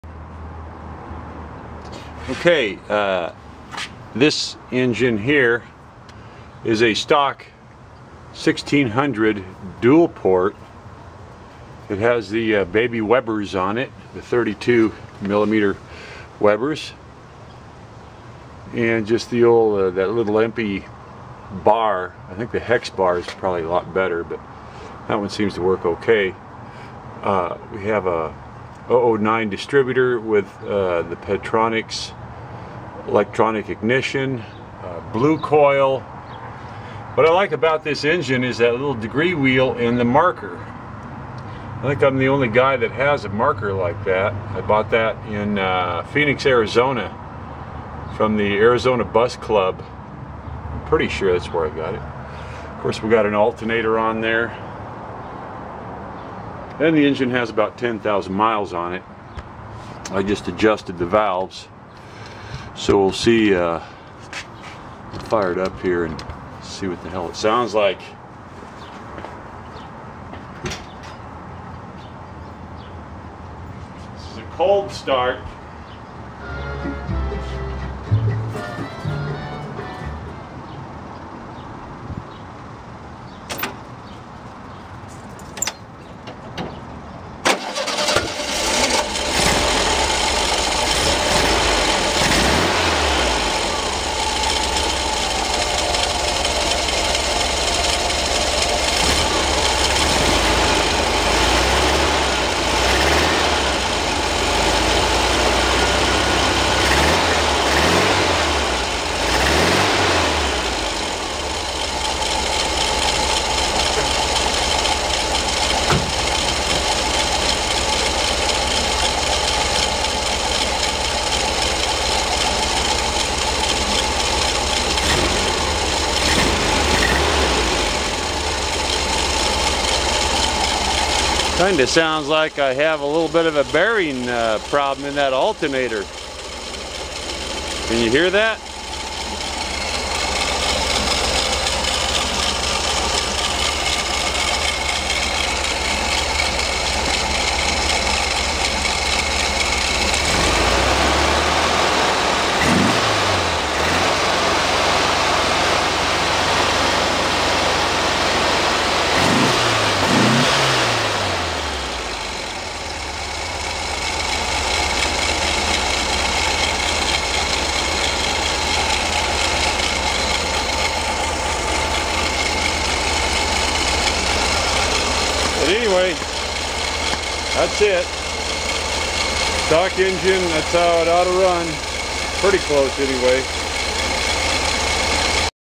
VW Karmann Ghia 1600 dual port
1974 Karmann Ghia vin number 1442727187 claimed to be the last Ghia standing or newest production number left alive. rebuilt 1600DP new case, dual Weber 32 carburetors, 009 distributor, Pertronix ignition, round bar Empi linkage Bosch Alternator, new KYB gas adjust shocks, degree pulley and a one off degree pointer makes it easy to set timing. handles great have had it up to 115MPH but the front end gets very light at high speeds. The little son of a bitch hauls ass and corners like it's on rails. Great way to get around town in a hurry. Daily driver. Thanks to Fernande Porsche for building the peoples car.

***PLEASE tell me if your Ghia has a newer production Vin number***